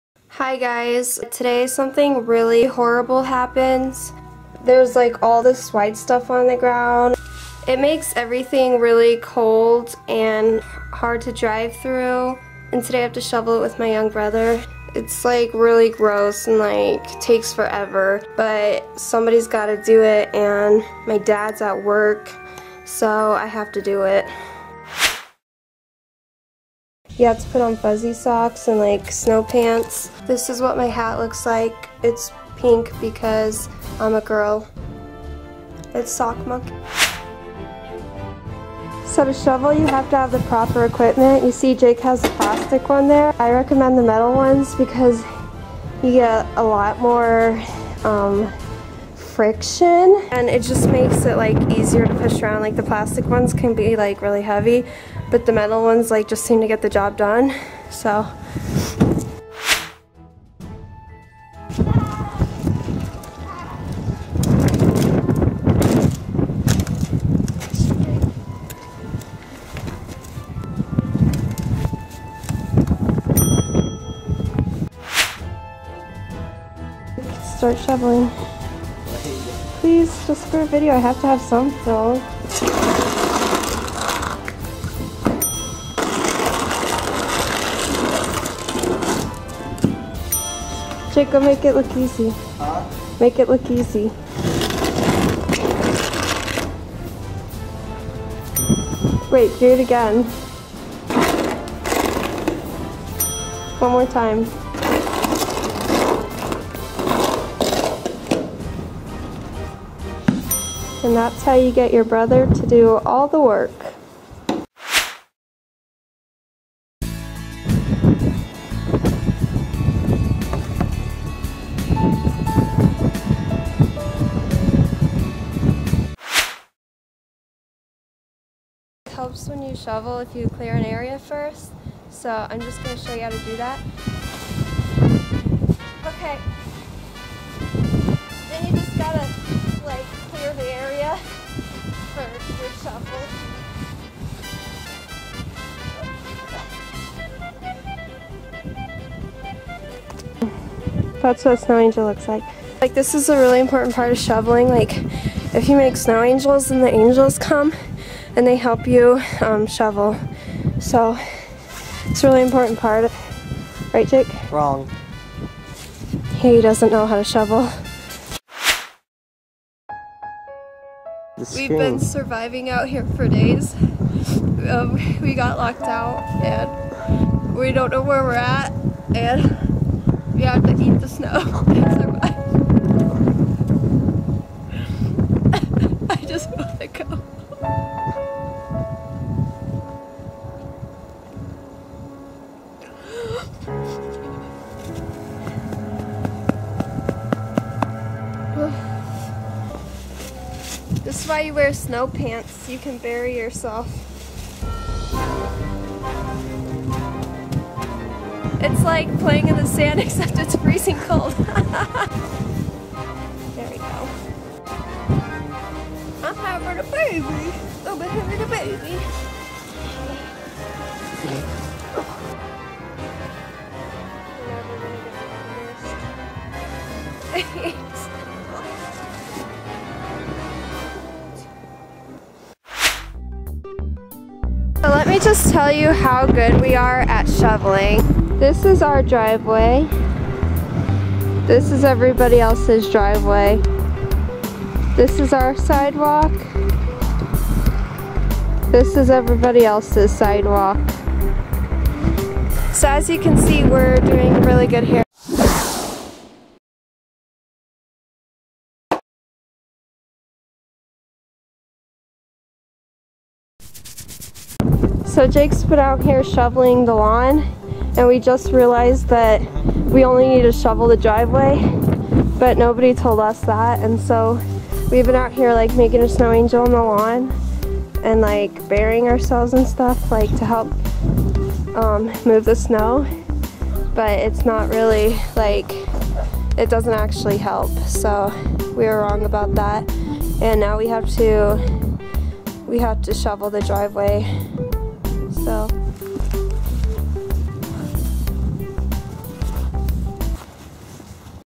Children Get Stuck Outside Shoveling
Forced to clean up snow, one child and his adult sister question why they weren't born in a beach community. Fear, hatred, insanity.. the snow has a huge effect on their mental well-being. Will they make it out alive? Watch to find out. Also watch to learn how to shovel if you wanted to learn that too. Because the girl will teach you what to wear and what to use and all the techniques--in the most proper manner.